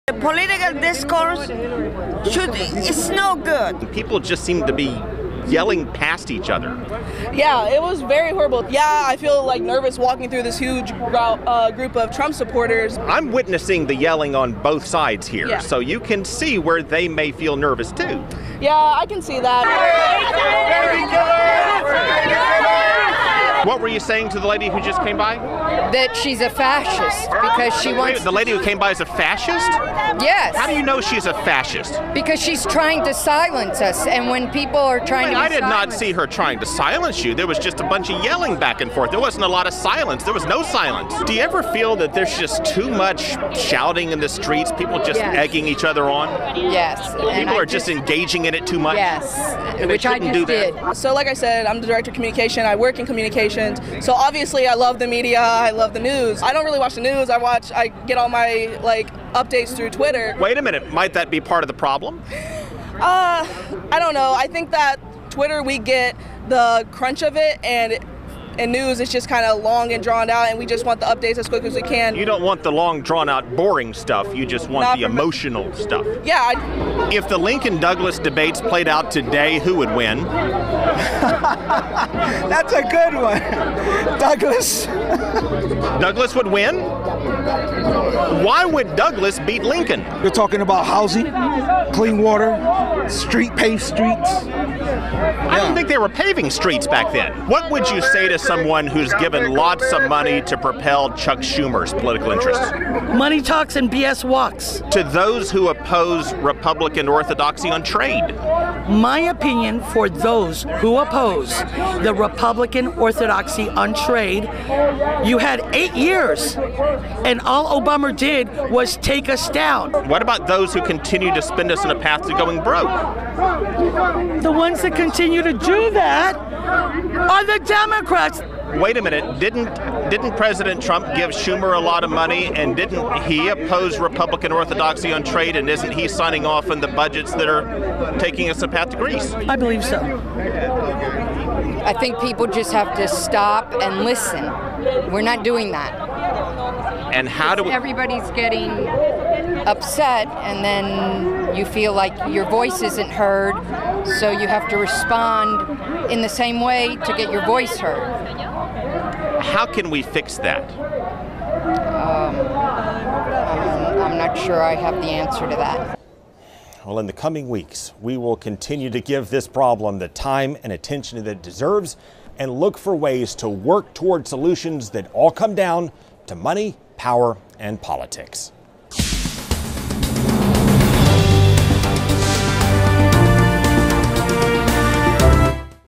The collapse of civility in American politics
America's crisis: a rise of ignorance and collapse of civility. When schools stop teaching civics, and people stop reading and watching news, and social media & wingnut commentators deliver the only stuff most people see-- this is what you get! (Note: in the course of meeting the protestors protesting the protestors in Miami, I found many had no idea of what they were even protesting, had little or no knowledge of current events, could not articulate views of the politicians they support and oppose, and had no idea of how our government works. I DID NOT include any of those people in our on-air interviews. Instead, I selected people from both sides of the aisle who appeared to have a higher level of knowledge about the system, and the problems we face).